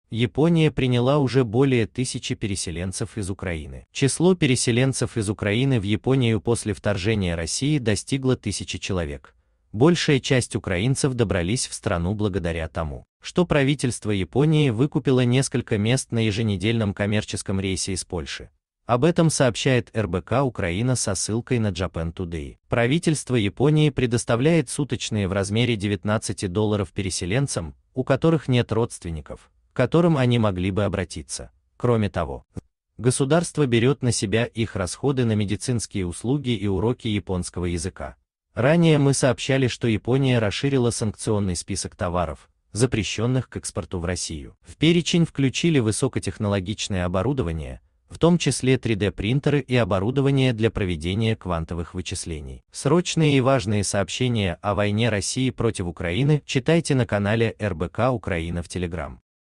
Япония приняла уже более тысячи переселенцев из Украины.
Япония Беженцы ВторжениеРоссиивУкраину Число переселенцев из Украины в Японию после вторжения России достигло тысячи человек. Большая часть украинцев добрались в страну благодаря тому, что правительство Японии выкупило несколько мест на еженедельном коммерческом рейсе из Польши. Об этом сообщает РБК-Украина со ссылкой на Japan Today. Правительство Японии предоставляет суточные в размере 19 долларов переселенцам, у которых нет родственников, к которым они могли бы обратиться. Кроме того, государство берет на себя их расходы на медицинск